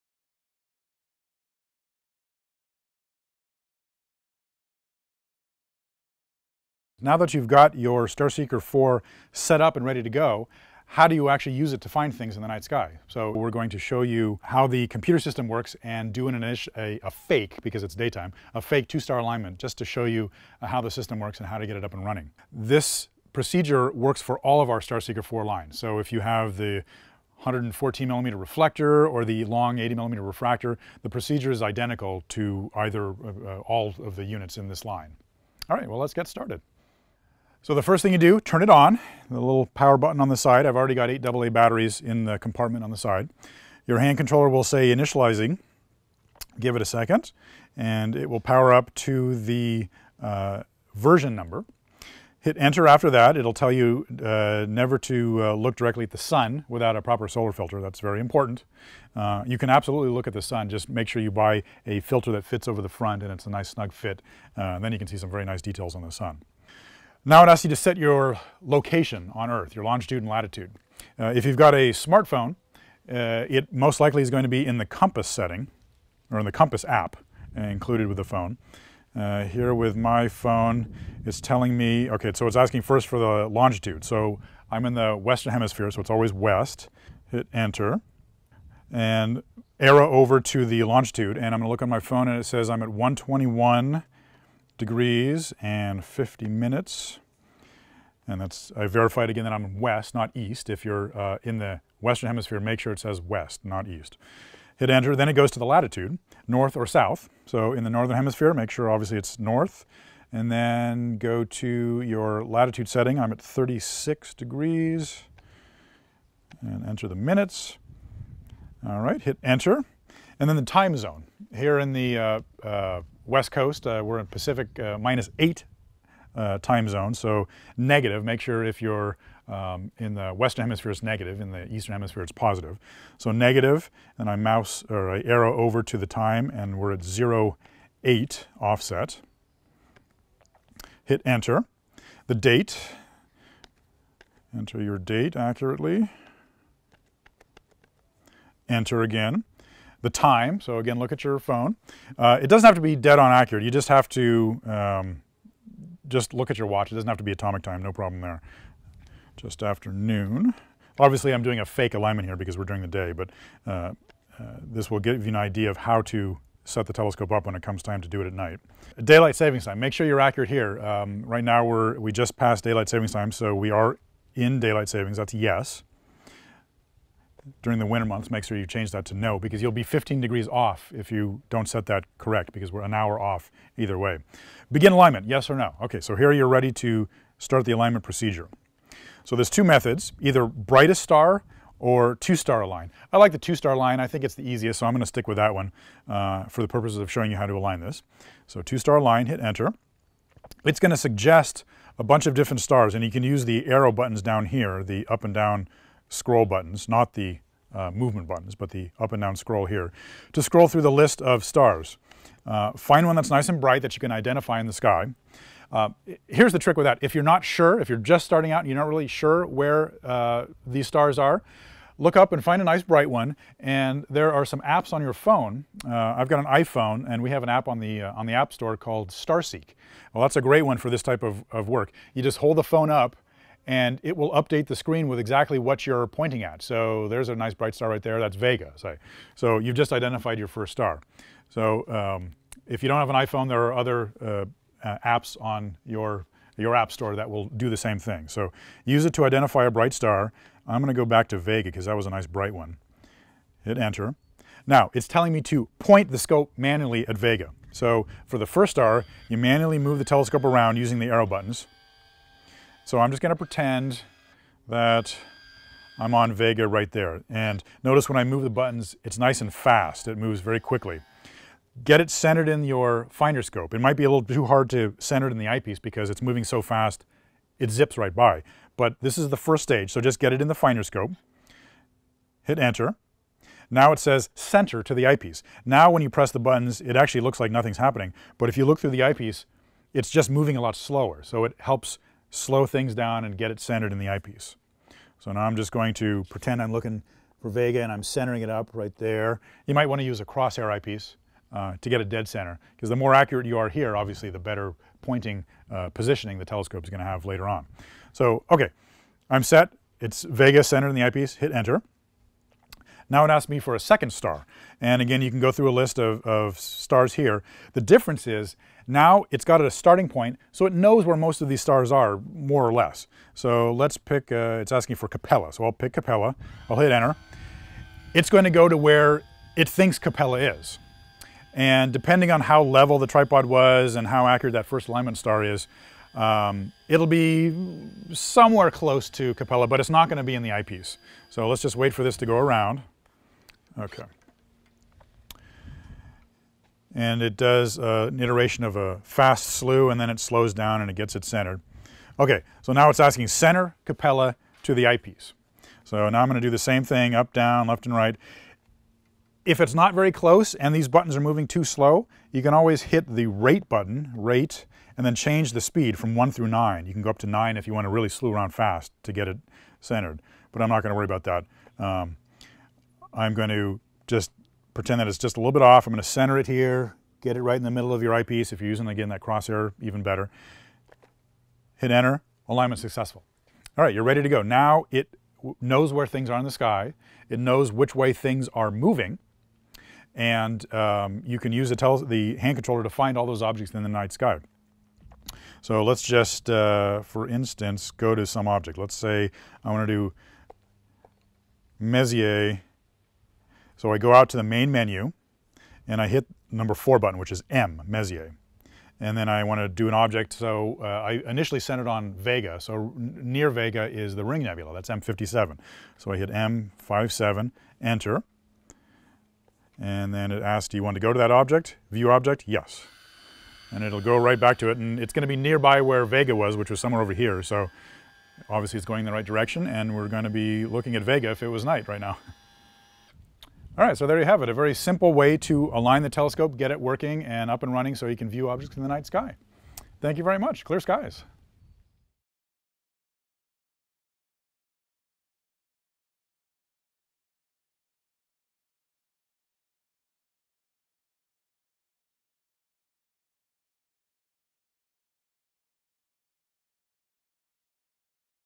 How to use an Orion StarSeeker IV GoTo Telescope - Orion Telescopes
This video demonstrates how to use the StarSeeker IV GoTo Telescope to find objects in the night sky.